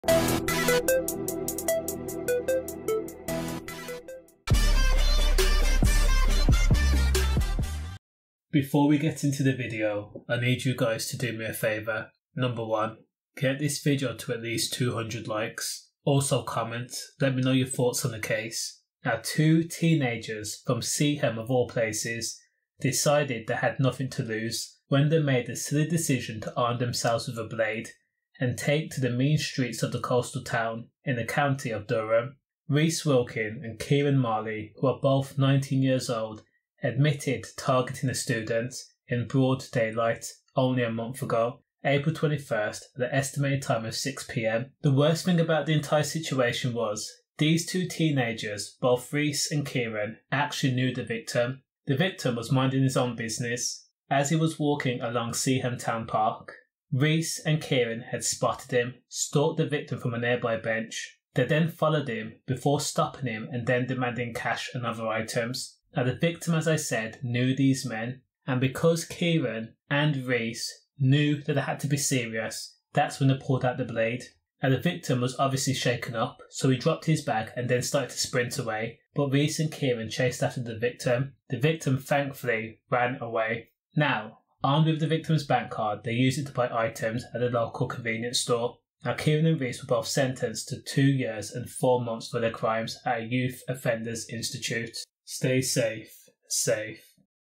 2 teenagers jailed after robbing a mate at kn*fepoint in broad daylight
Two teenagers will spend two years each behind bars after admitting robbing a student in broad daylight last month.

Reece Wilkin and Kieran Marley, both 19, admitted robbing their victim at the entrance to Seaham Town Park shortly after 6pm on April 21 this year. Wilkin also admitted fraud and possession of a blade